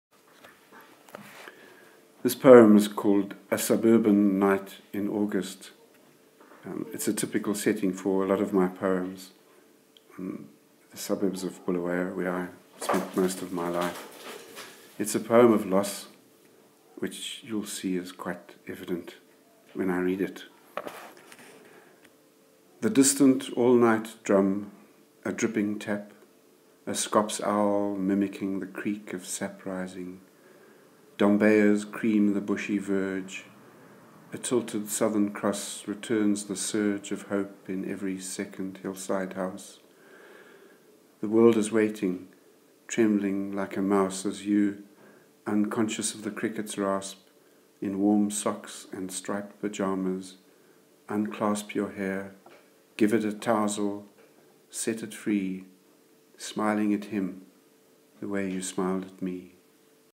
A Suburban Night in August - John Eppel
John Eppel reads 'A Suburban Night in August' from 'Textures, poems by John Eppel and Togara Muzanenhamo', launched in Bulawayo in March 2015 at CBC and at Indaba Book cafe